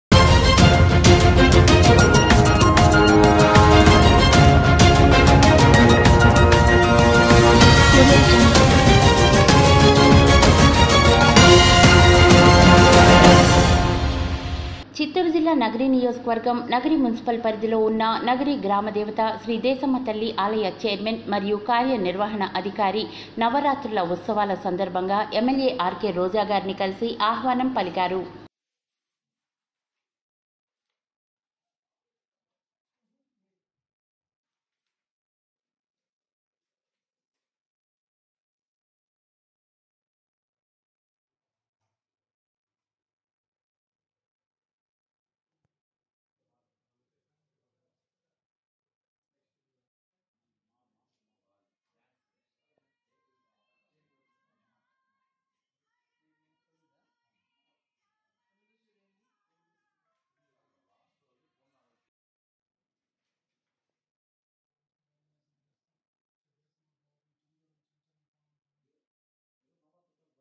నవరాత్రుల ఉత్సవాల సందర్భంగా ఎమ్మెల్యే ఆర్కే రోజా | SAKSHITHA NEWS
చిత్తూరు జిల్లా నగరి నియోజకవర్గం
నగరి మునిసిపల్ పరిధిలో నున్న
నగరి గ్రామదేవత శ్రీ దేశమ్మ తల్లి ఆలయ చైర్మన్ మరియు కార్యనిర్వహణ అధికారి నవరాత్రుల ఉత్సవాల సందర్భంగా ఎమ్మెల్యే ఆర్కే రోజా గారిని కలిసి ఆహ్వానం పలికారు